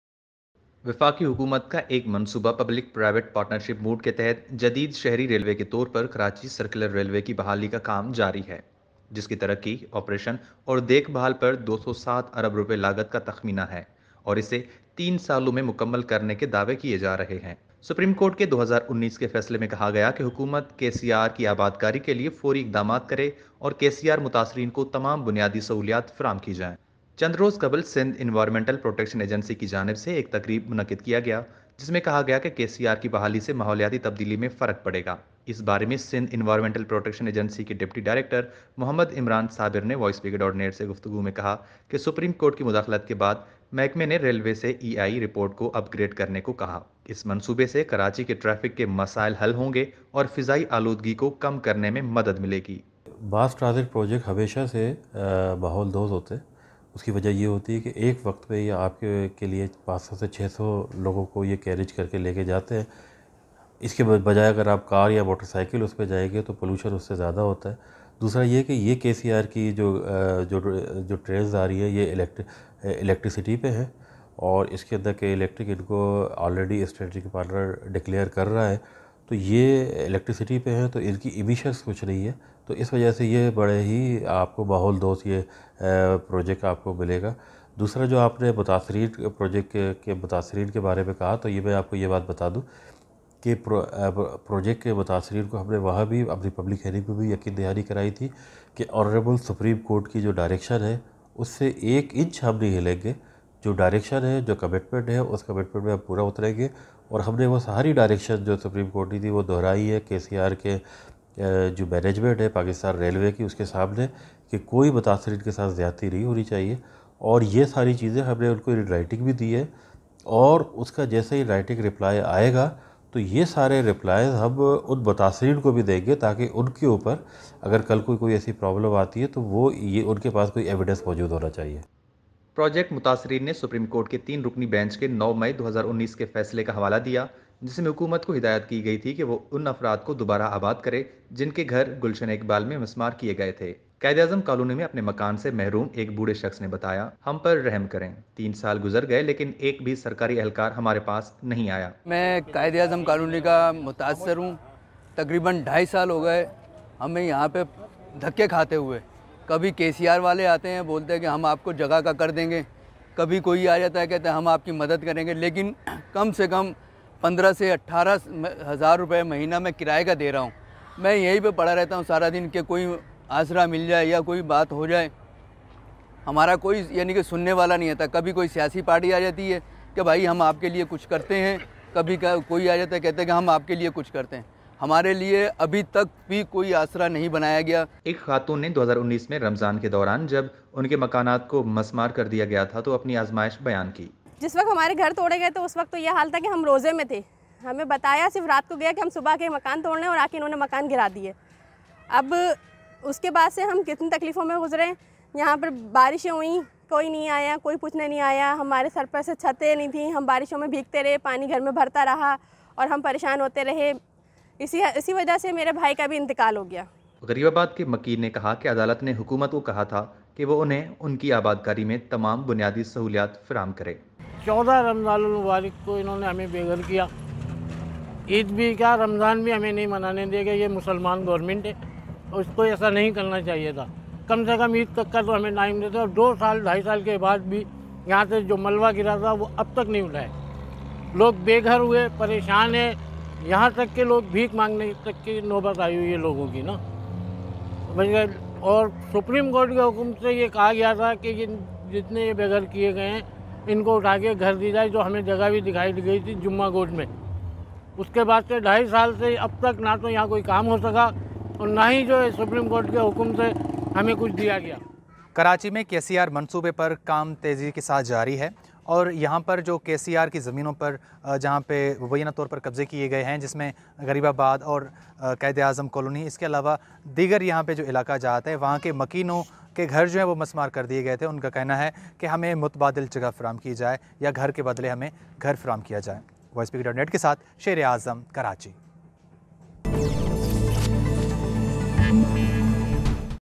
Residents displaced in KCR project decry govt apathy
Residents of several localities whose houses were demolished to pave way for the Karachi Circular Railway project await government compensation and resettlement as promised to them in the 2019 Supreme Court judgment